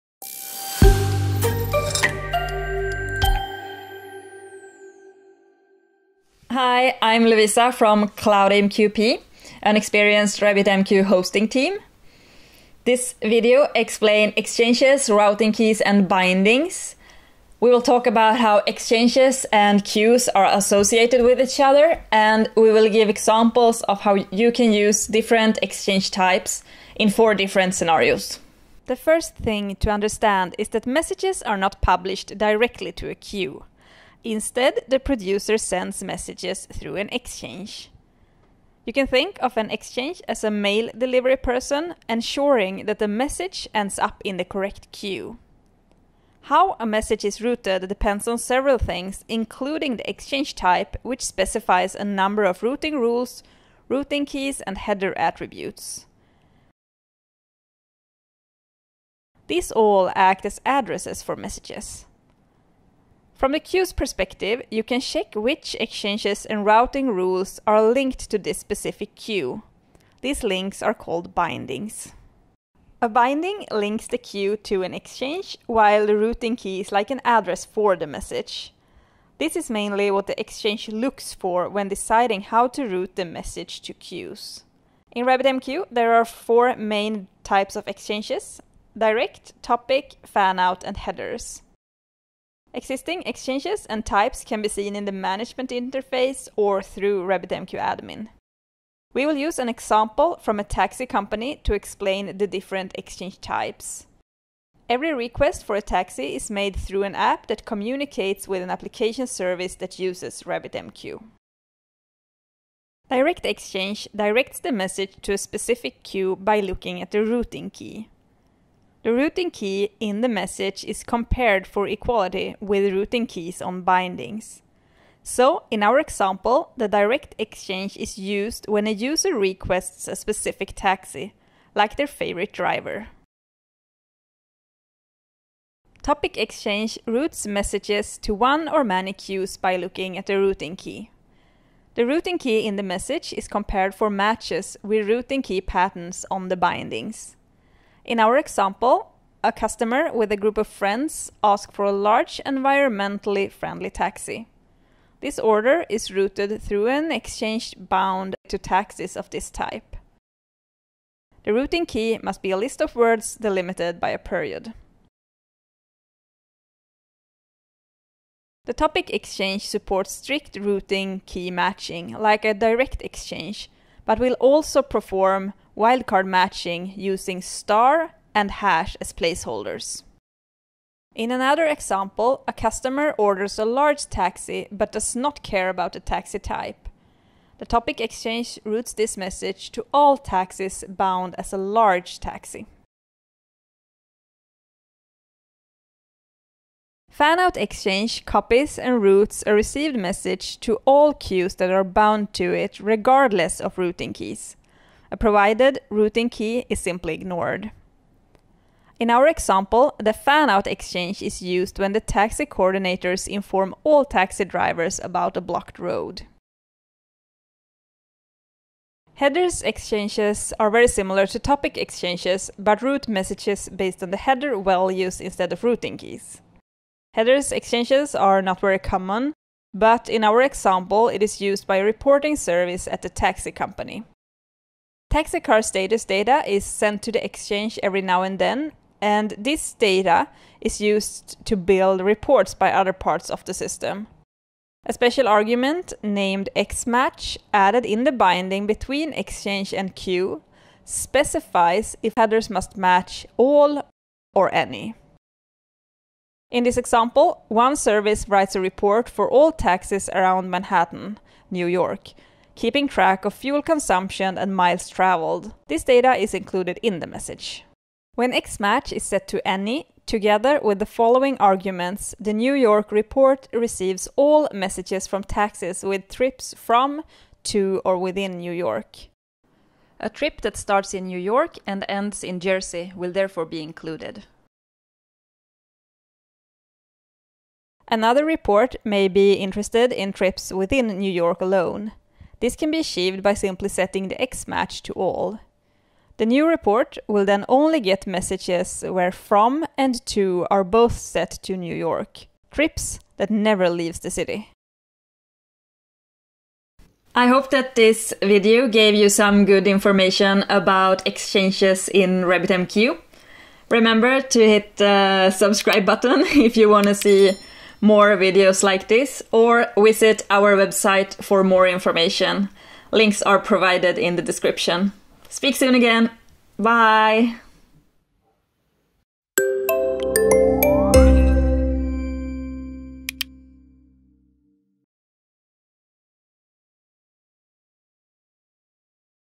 RabbitMQ Explained - Exchanges
Messages are not sent directly to Queues. They first pass through an Exchange (in the broker) that redirects them to their destination. This video explains the interactions between exchanges, routing keys and bindings in RabbitMQ.

We will talk about how exchanges and queues are associated with each other, and show examples of how you can use the different exchange types in four different scenarios.

In the video we focus on the main types of exchanges available in RabbitMQ:
- Direct Exchange
- Topic Exchange
- Fanout Exchange
- Headers Exchange

Segments:
Intro: (0:00)
What is an Exchange?: (0:28)
Direct Exchange - Example: (1:38)
Topic Exchange - Example: (2:15)
Fanout Exchange - Example: (3:15)
Headers Exchange - Example: (3:38)
Outro: (5:06)

CloudAMQP is the largest hosting provider of the open-source message broker - RabbitMQ. Try it free

New to RabbitMQ? What is RabbitMQ?